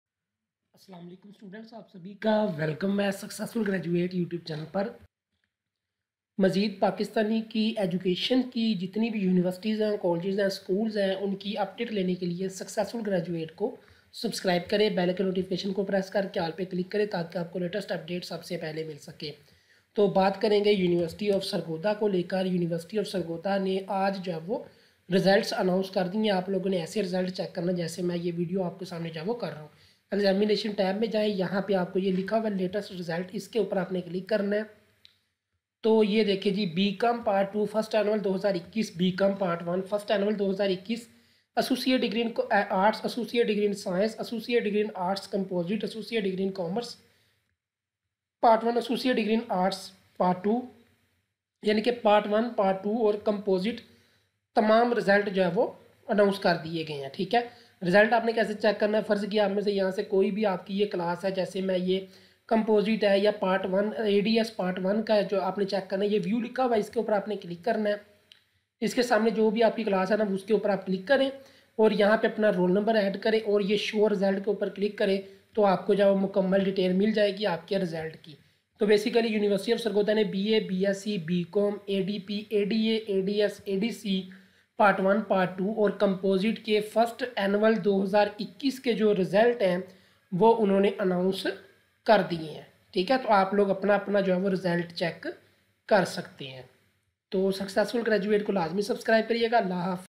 Ba Bsc BCom Ada Ads Ada Part 1 & 2 & Composite 1st Annual 2021 Results Announced Sargodha University
8th 5th
FA FSC ICS I.COM
11th 12th